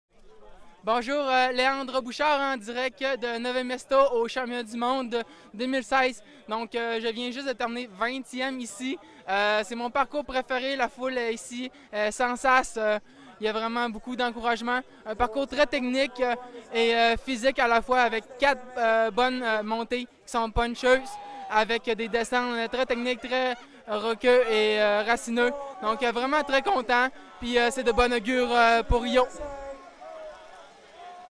2016 MTB Worlds Elite Men - Leandre Bouchard (French)
An interview with Leandre Bouchard, 20th in Elite men at the 2016 Mountain Bike World Championships